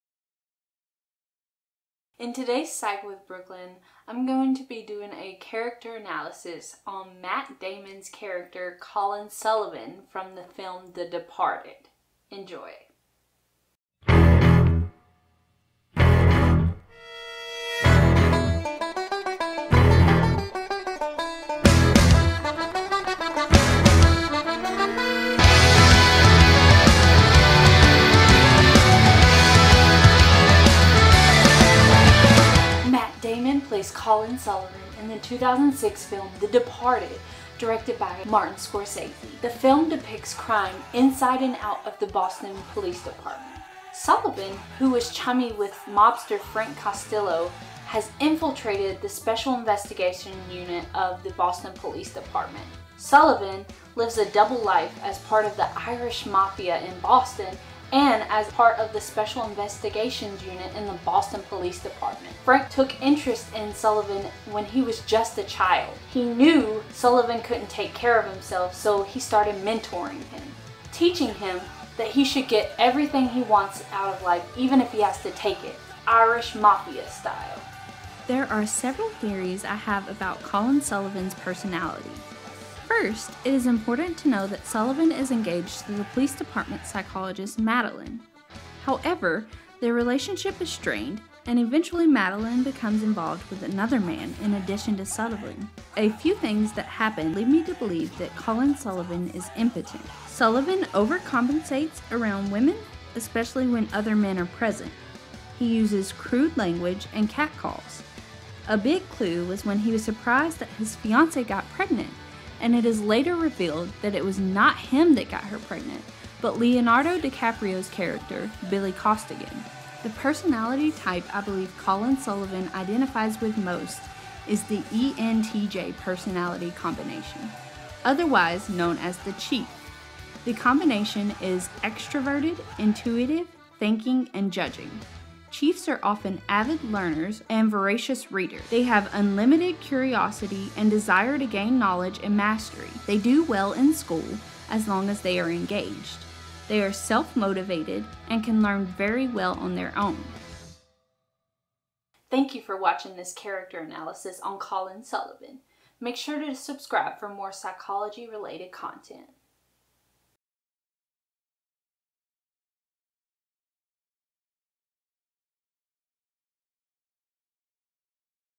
Character Analysis on Colin Sullivan from The Departed
Brooklyn gives a detailed character analysis on the character Colin Sullivan from the 2006 film The Departed.